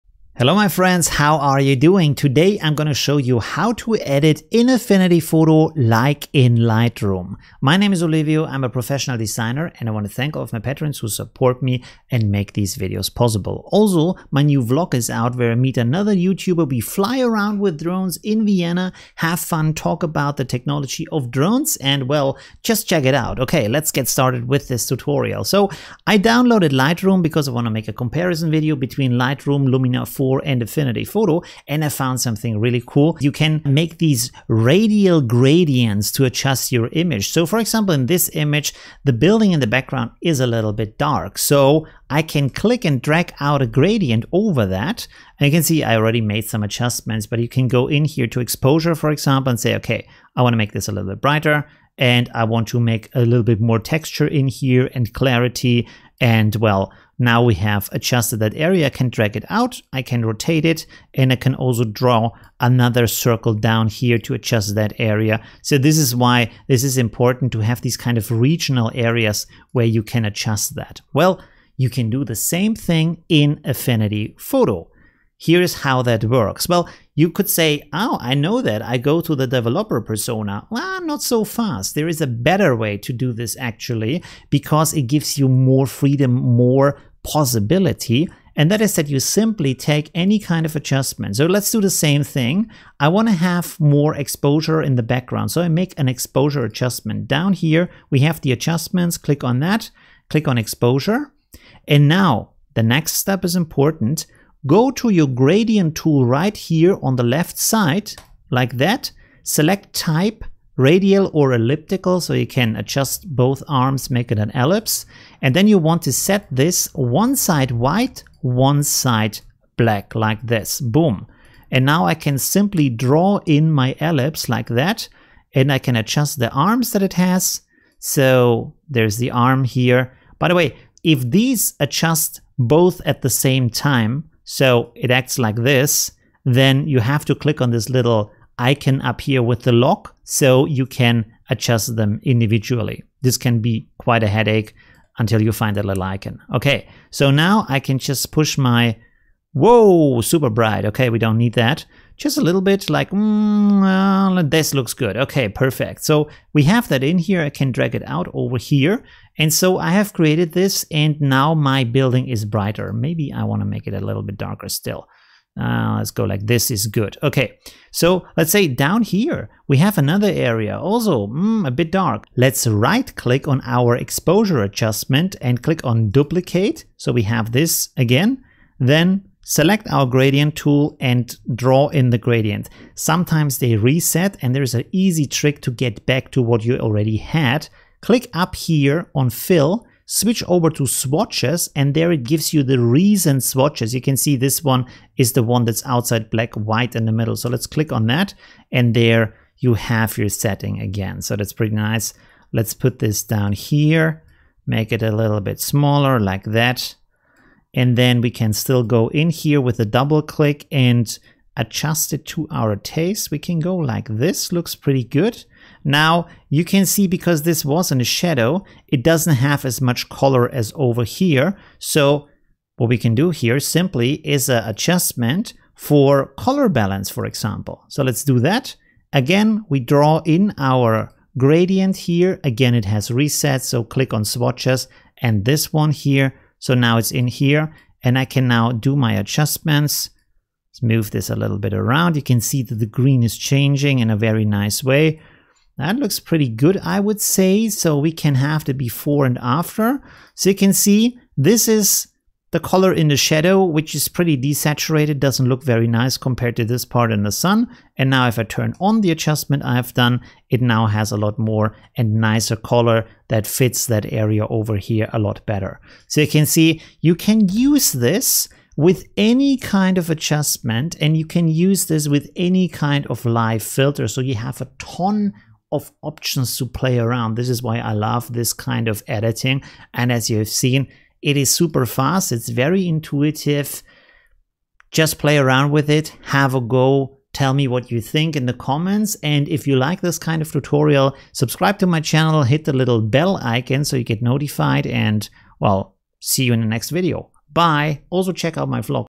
Edit like in Lightroom? - Affinity Photo Tutorial
Can you editing like in Lightroom? Yes! I will show you a very easy trick to edit as fast and intuitive as in Lightroom. And the best thing: it's all non-destructive. This simple use of Gradient Masks on Adjustment and Live-Filter Layers makes Affinity Photo extremely powerful.